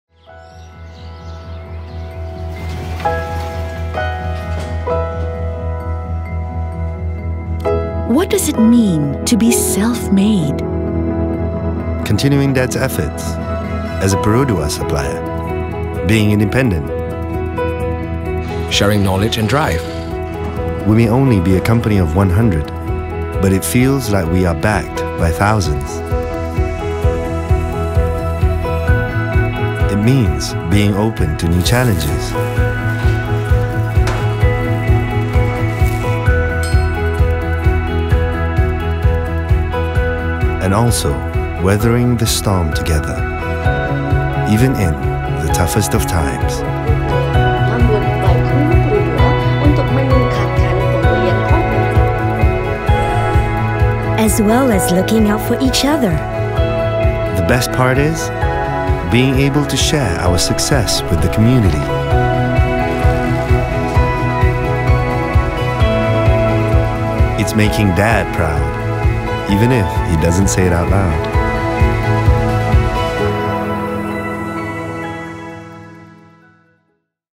We Go Beyond – Ecosystem
We don’t just mind our own business. Through knowledge sharing and investments, WeGoBeyond to raise local suppliers to global standards, to help them compete on the world stage and boost the nation’s economy.